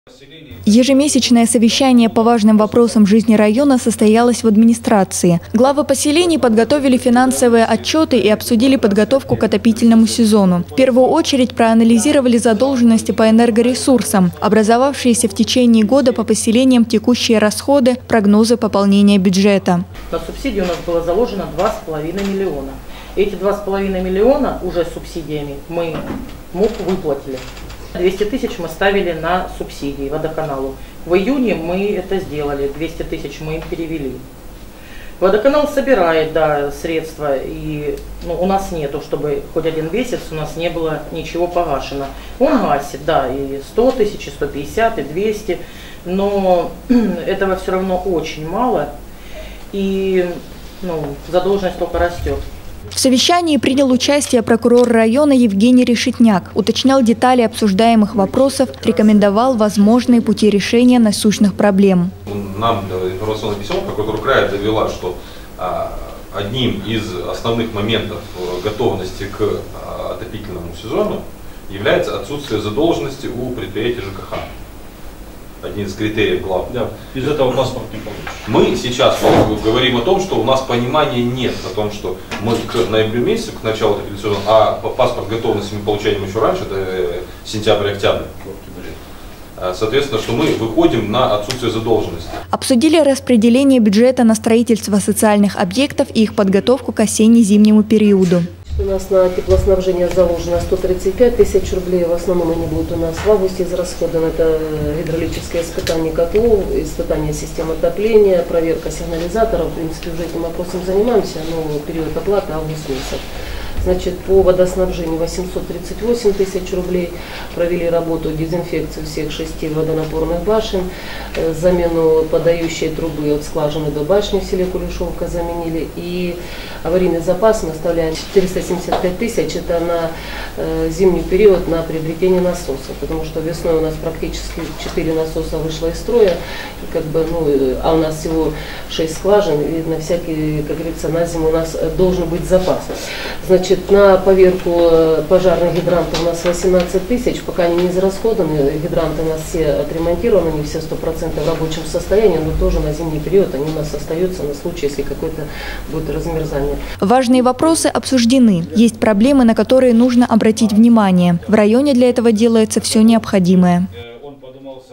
Совещание по подготовке к отопительному сезону
Ежемесячное совещание по важным вопросам жизни района состоялось в администрации. Главы поселений подготовили финансовые отчеты и обсудили подготовку к отопительному сезону. Смотрите подробнее в нашем сюжете